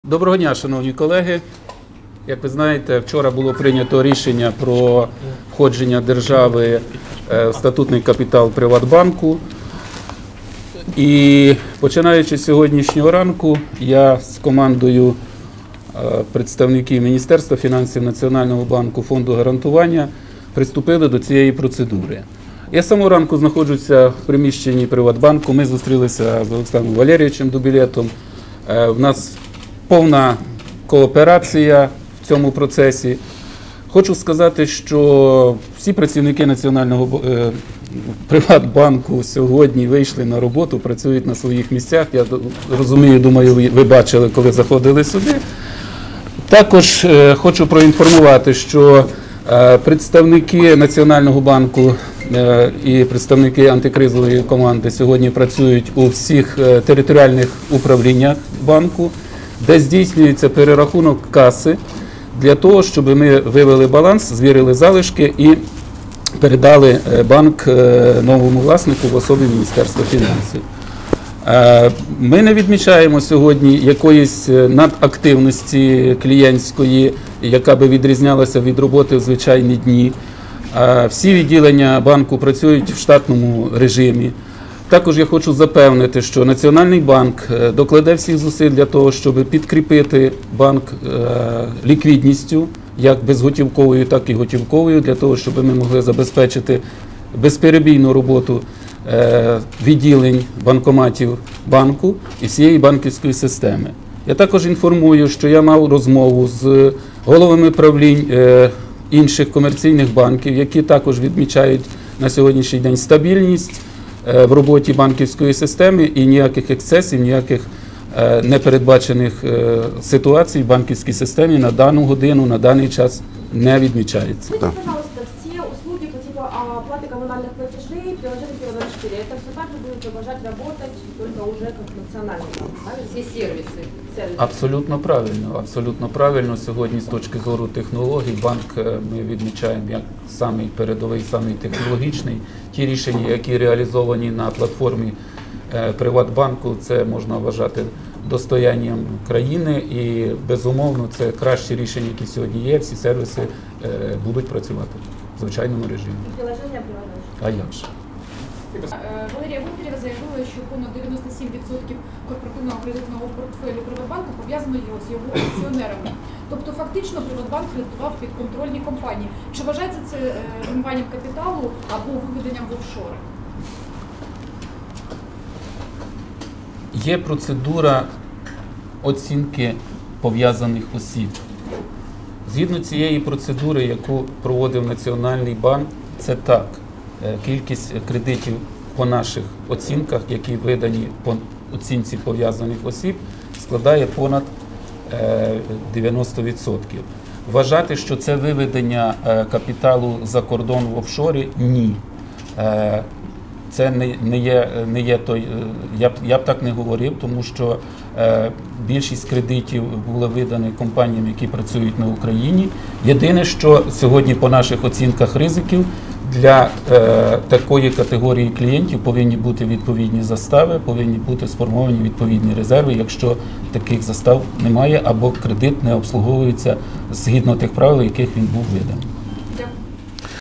Виступ першого заступника Голови НБУ Якова Смолія під час брифінгу щодо поточної роботи Приватбанку
19 грудня 2016 року
місто Дніпро